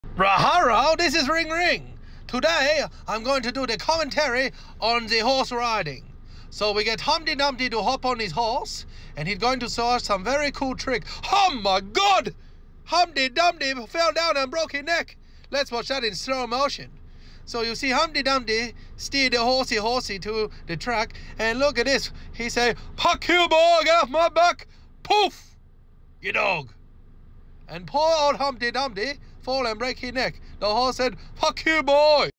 Ling ling and the horse riding
Ling ling is back, commentating the horse riding